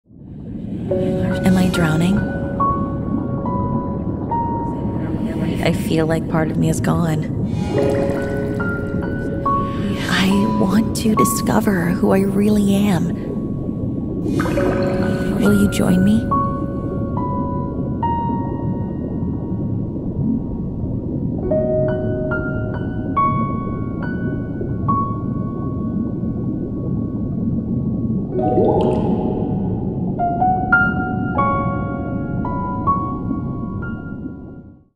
UMI'S UNDERWATER ADVENTURE【DEBUT PV】
Who am I? Who was I supposed to be? Am I... alone?

Credits:
✦ Art: negi_toro07
✦ SFX: Jays_TreeDee
✦ Logo: AmatsukiLuni
✦ Voice: Me!

I'm Umi, helping you find your peace within the current of the ocean 🌊💤

⋆｡ ﾟ☁︎｡ ⋆｡ ﾟ☾ ﾟ｡ ⋆